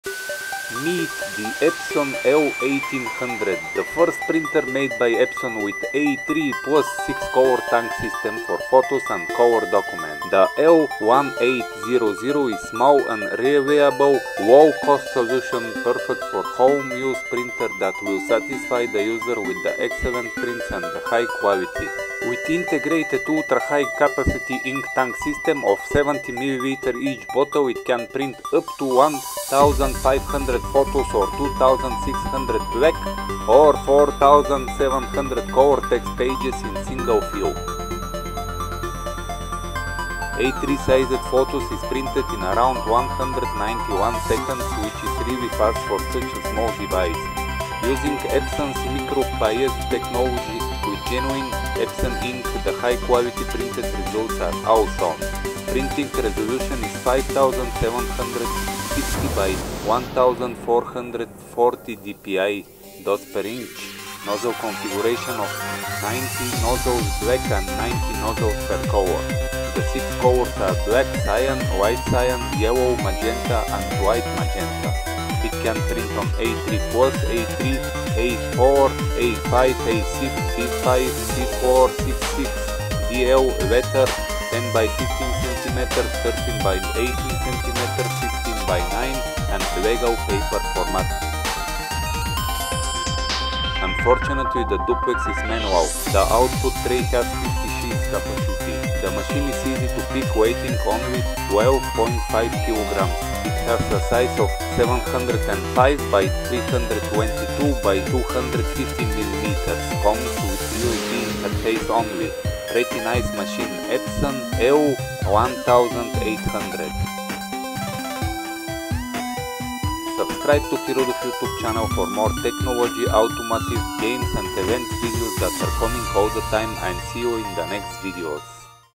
Epson L1800 printer review in 3D
Fast review of Epson L1800 printer in 3D.

Specs of the printer:
TECHNOLOGY
Printing Method: On-demand inkjet (Piezo electric)
Minimum Droplet Size: 1.5 pl, With Variable-Sized Droplet Technology
Ink Technology: Dye Ink
Printing Resolution: 5,760 x 1,440 dpi
Nozzle Configuration: 90 Nozzles black, 90 Nozzles per colour
Application: Home, Photo
Printing Speed: ISO/IEC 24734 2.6 Pages/min Monochrome, 2.6 Pages/min Colour
Maximum Printing Speed: 15 Pages/min Monochrome (plain paper), 15 Pages/min Colour (plain paper), 45 Seconds per 10 x 15 cm photo (Epson Premium Glossy Photo Paper)
Colours: Black, Cyan, Light Cyan, Yellow, Magenta, Light Magenta
Number of paper trays: 1
Paper Formats: A3+, A3, A4, A5, A6, B5, C4 (Envelope), C6 (Envelope), DL (Envelope), Letter, 10 x 15 cm, 13 x 18 cm, 16:9, Legal
Duplex: Manual
Paper Tray Capacity: 100 Sheets Standard, 30 Photo Sheets
Media Handling: Manual duplex
Output Tray Capacity: 50 Sheets
Product dimensions: 705 x 322 x 215 mm (Width x Depth x Height)
Product weight: 12.5 kg
Compatible Operating Systems: Mac OS 10.5.8 or later, Windows 7, Windows 8, Windows Vista, Windows XP
Supply Voltage: AC 110 V - 240 V,50 Hz - 60 Hz
Interfaces: USB
Power Supply: 100V, 110V, 220V, 240V